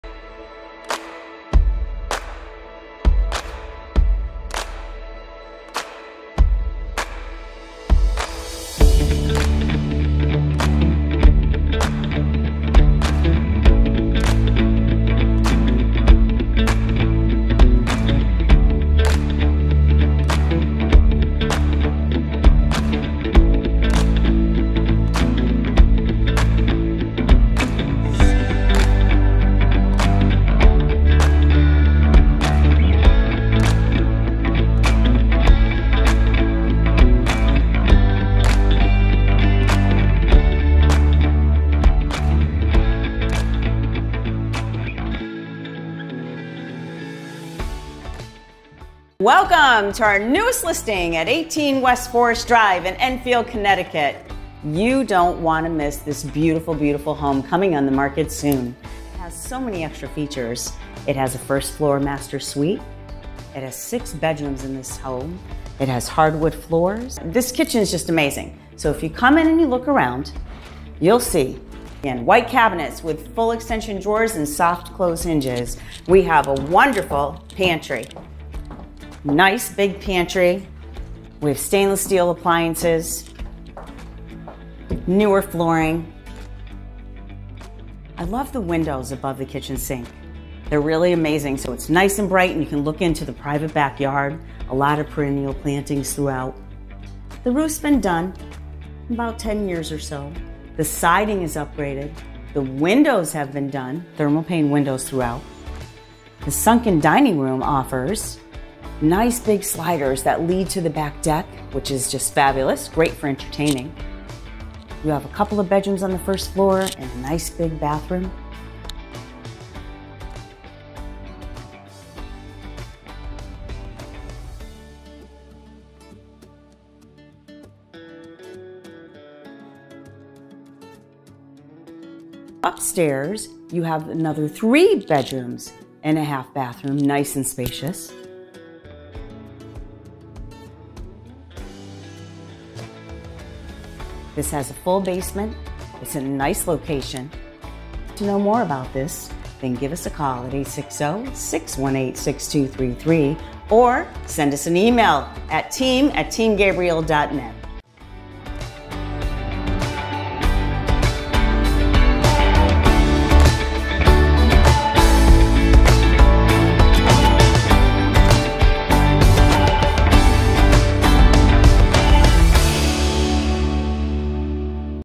Open House - 18 West Forrest Drive, Enfield, CT
*OPEN HOUSE This THURSDAY!*
18 West Forrest Drive, Enfield, CT
May 18, 2023 From 5-7 PM

Stop by this THURSDAY May 18, 2023 from 5 - 7 PM  For an Open House!

Fabulous One Owner Home! Enjoy this spacious Colonial with 2,092 SF and a RARE OPPORTUNITY to spread out in this 6 Bedroom, 2 1/2 Bathroom Home! The Kitchen has been remodeled, with White soft close hinged Cabinetry, pantry space, Stainless Steel Appliances, new Flooring and Granite Countertops. Open to the Sunken Dining area with sliding glass doors that lead to an expanded deck overlooking the very private large yard filled with perennials.

We Hope To See YOU This THURSDAY, May 18, 2023 from 5 - 7 PM at 18 West Forrest Drive, Enfield, CT!  Come By & Say Hi!

Edited By: Erla Joy Escarcha

Looking to buy a home in the North Central CT, Western MA or South Florida area?

Looking to sell a home? Find out the value of your home